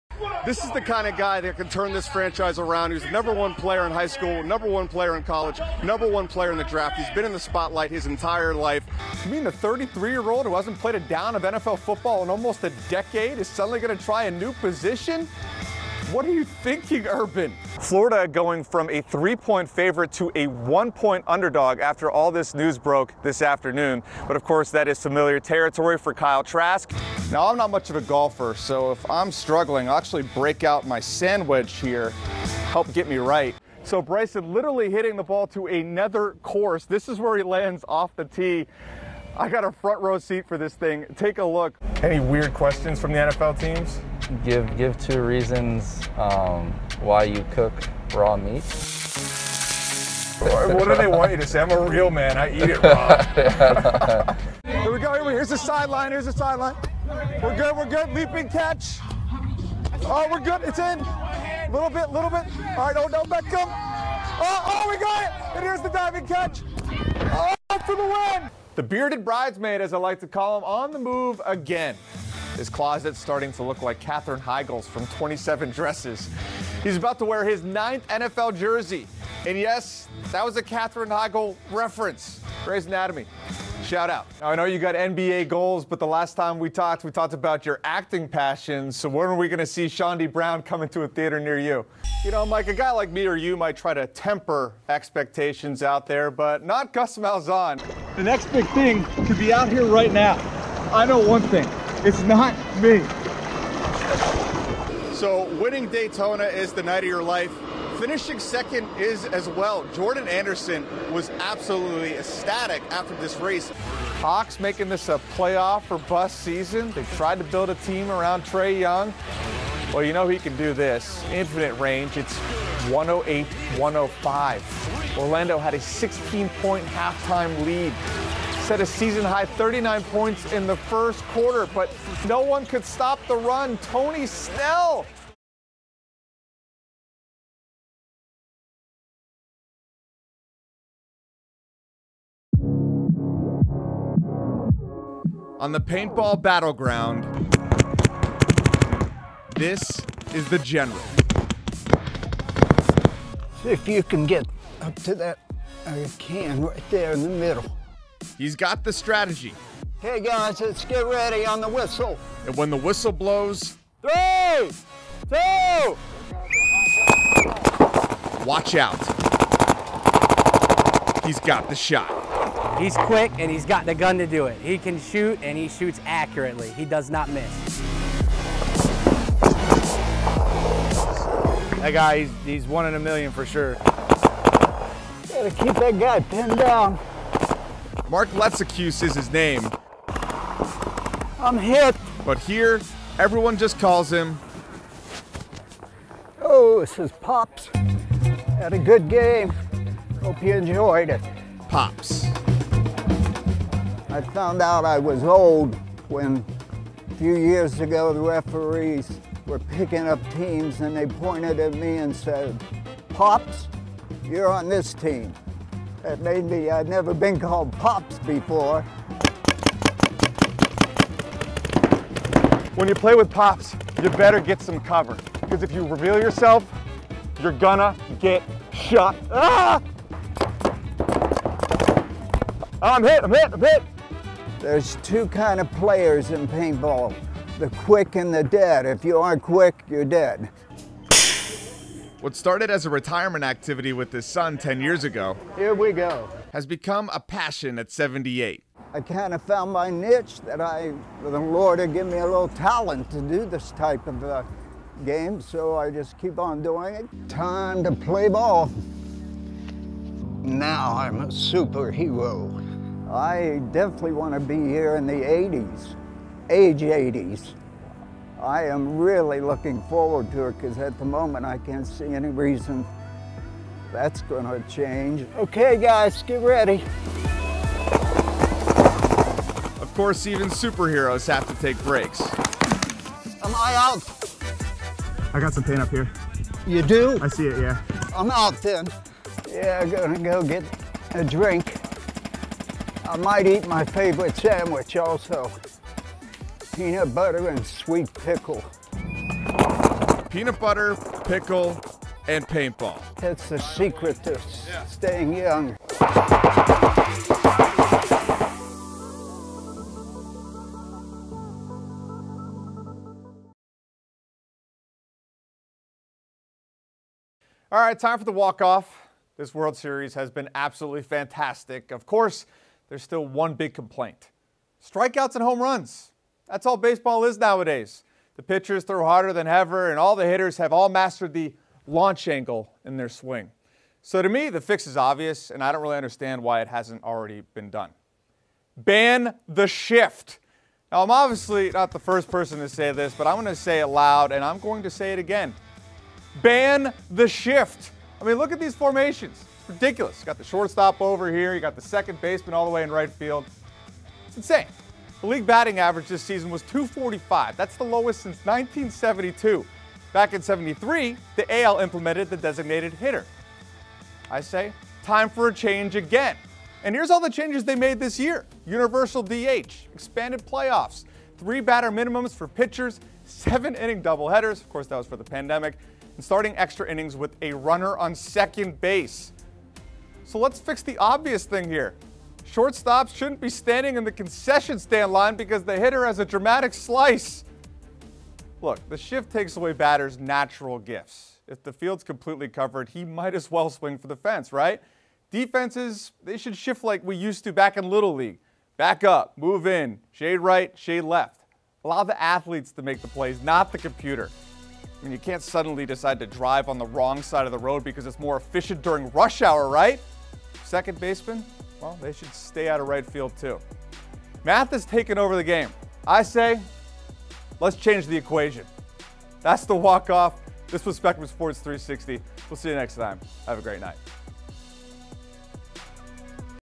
Pat Welter Sports Anchor/MMJ Reel May 2021 update
Pat Welter covers sports for Spectrum News 13 and Spectrum Sports 360 in Orlando, Fl.